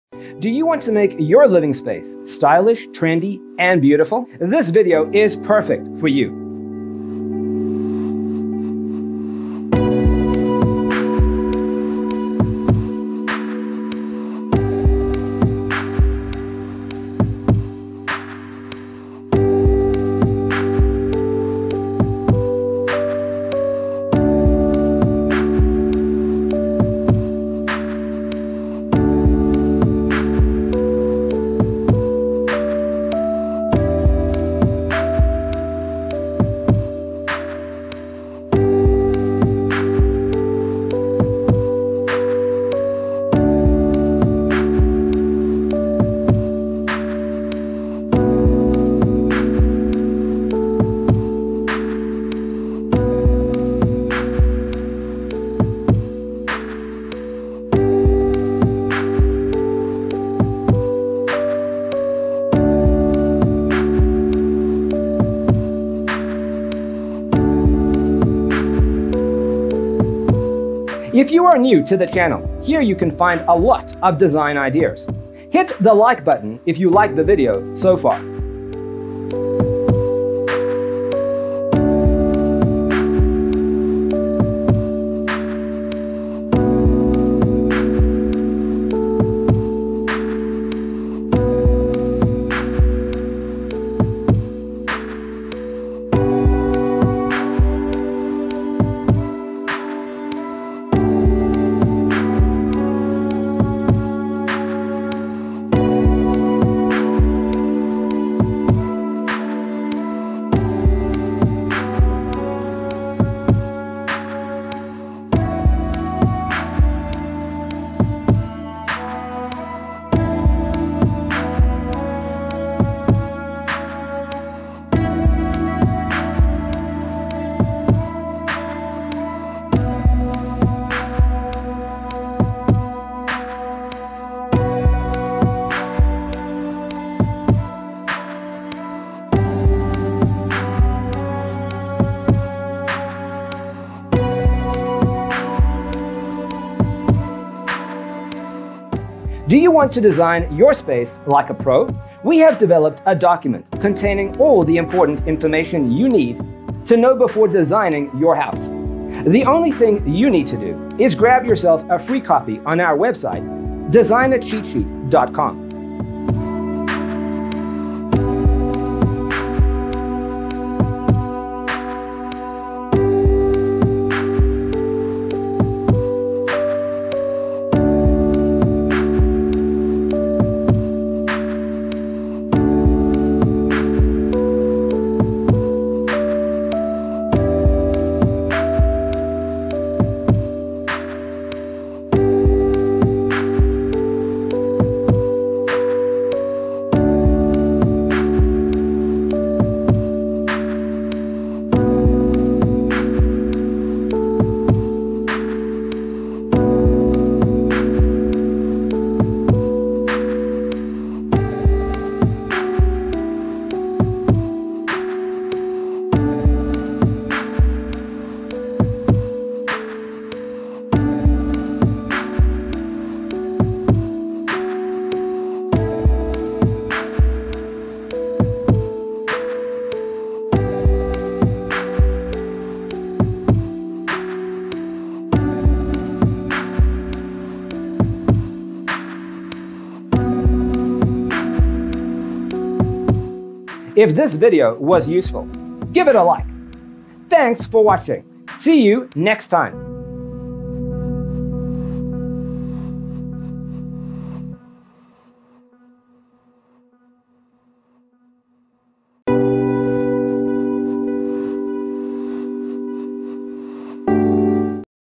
75 Southwestern Orange Bedroom Design Ideas You'll Love ♡
Browse southwestern bedroom decorating ideas and layouts. Discover bedroom ideas and design inspiration from a variety of southwestern bedrooms, including color, decor and theme options.

0:00:00 - Intro
0:00:12 - Southwestern Orange Bedroom Design Ideas You'll Love: Inspirations 1 to 25
0:01:23 - Southwestern Orange Bedroom Design Ideas You'll Love: Inspirations 26 to 50
0:02:45 - Southwestern Orange Bedroom Design Ideas You'll Love: Inspirations 51 to 75